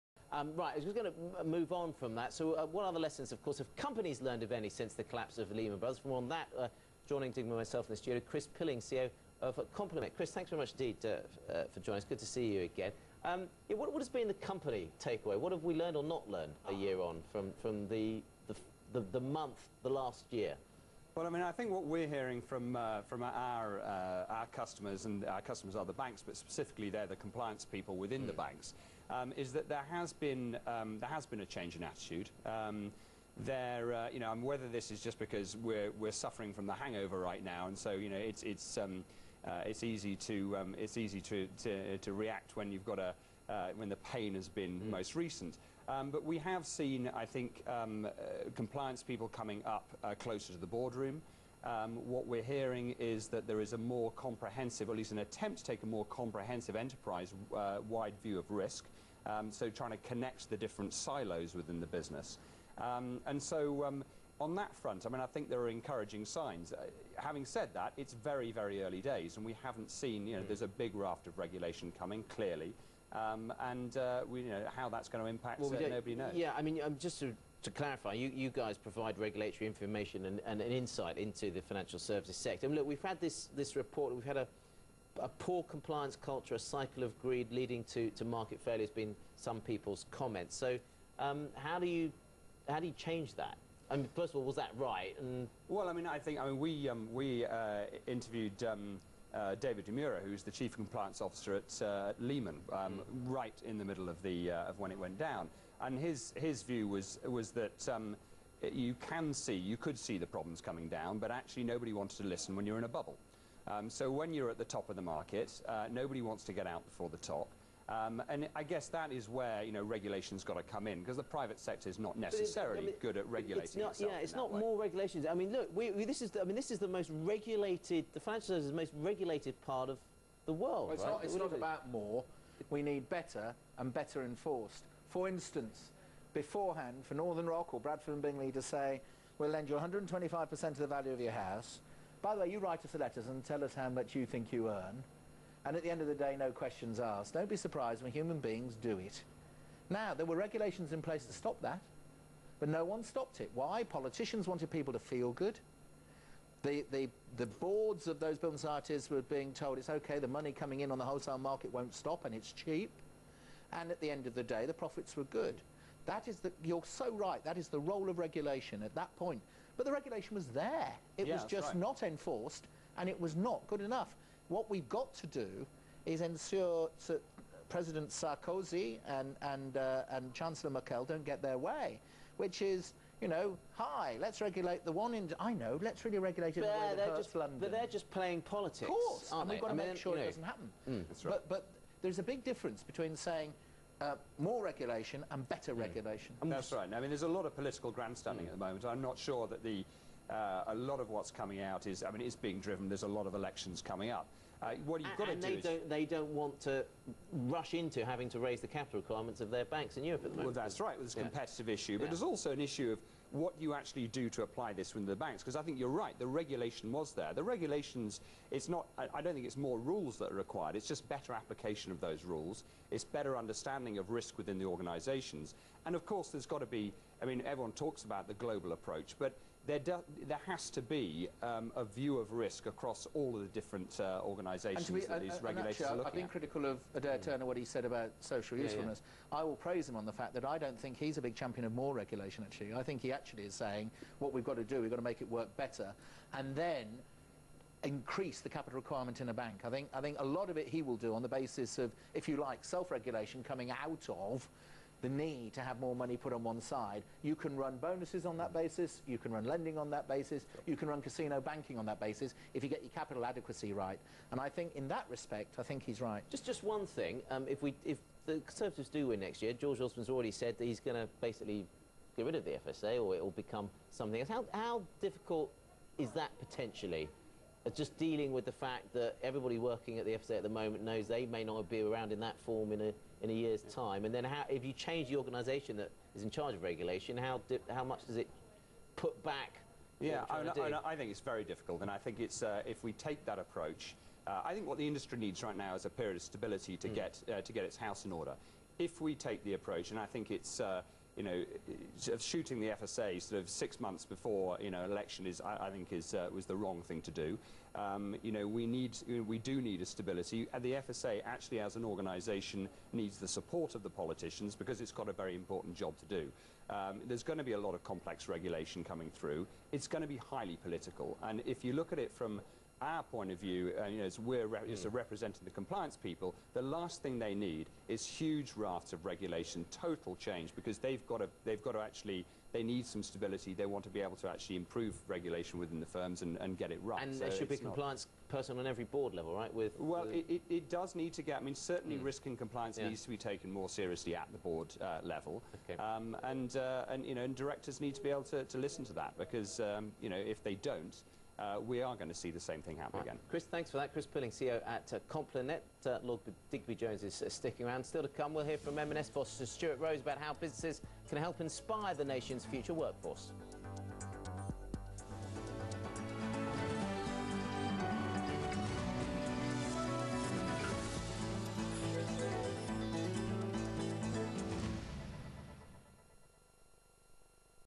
Compliance at the Forefront
Compliance issues have become more important to companies since the collapse of Lehman Brothers last year, Chris Pilling, CEO of Complinet, told CNBC Tuesday.